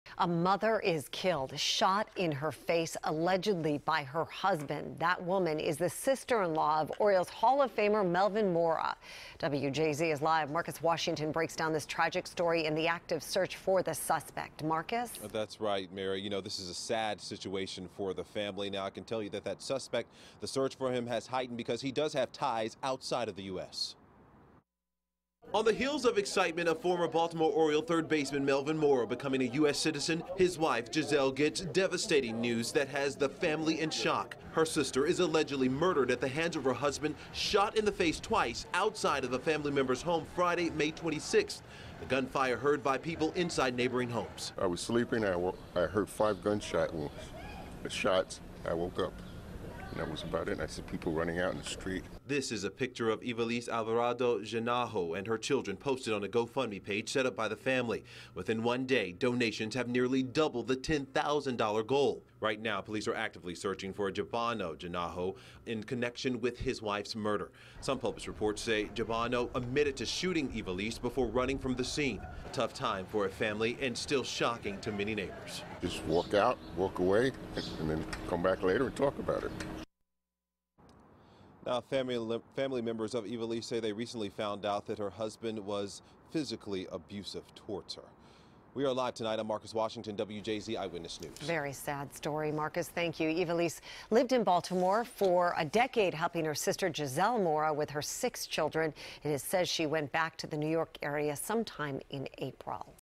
Sister-In-Law Of Former Oriole Allegedly Murdered By Husband
Marcus Washington reports.